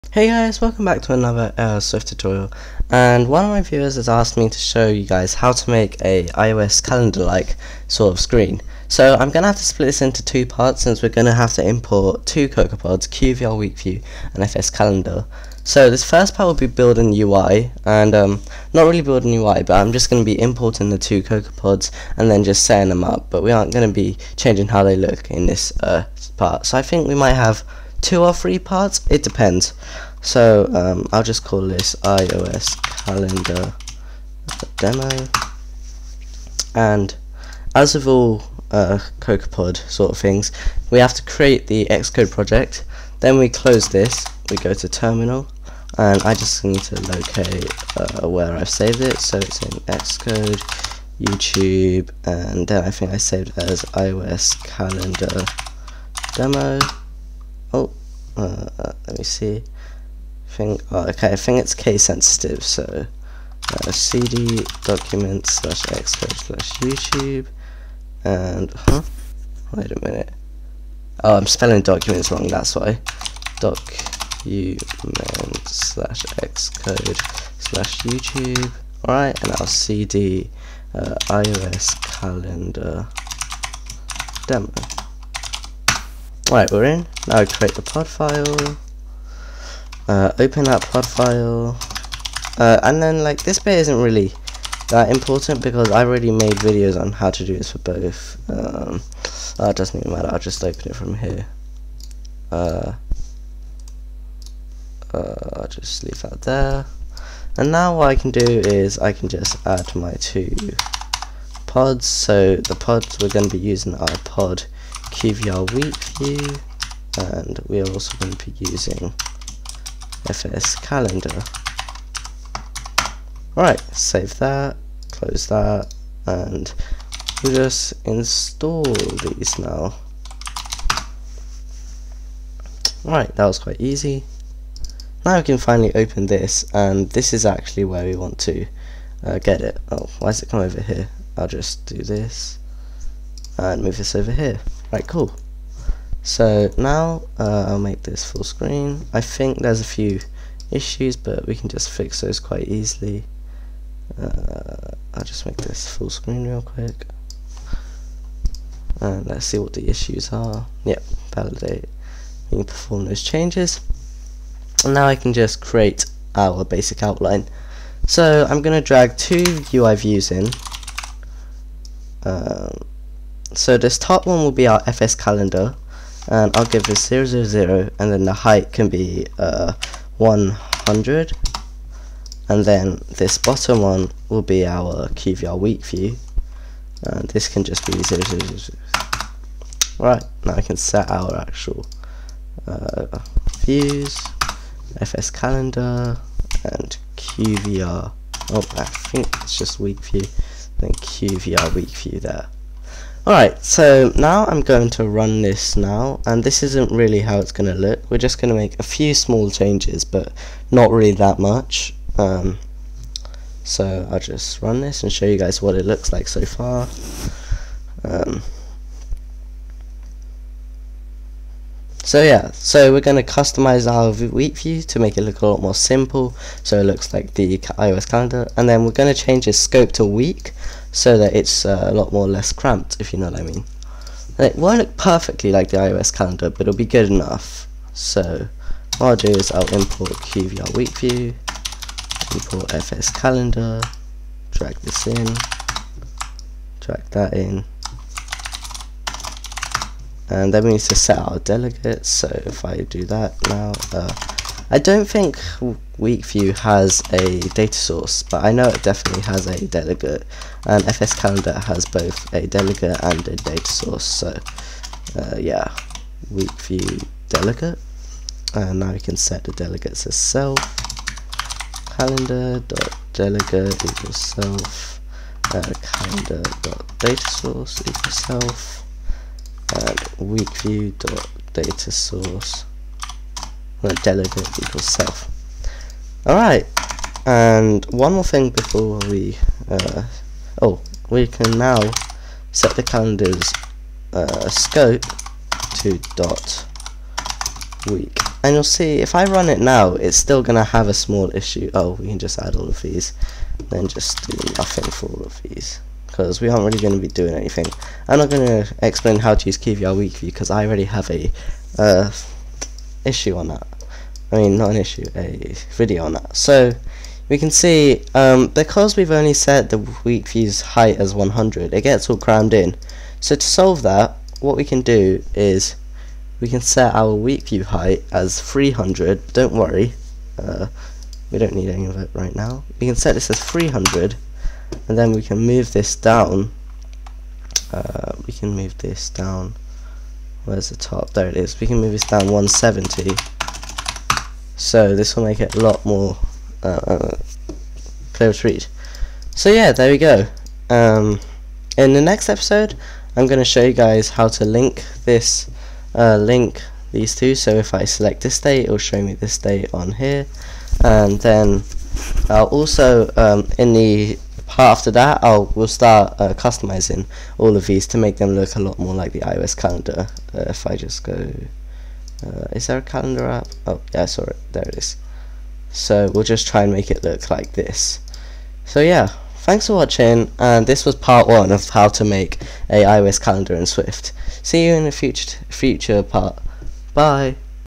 How to create an iOS like Calendar P1
In this tutorial I will begin a new series to show you how to create a calendar using QVRWeekView and FSCalendar, this this part I am just going to import the 2 cocoapods and set out the foundations for making it look nice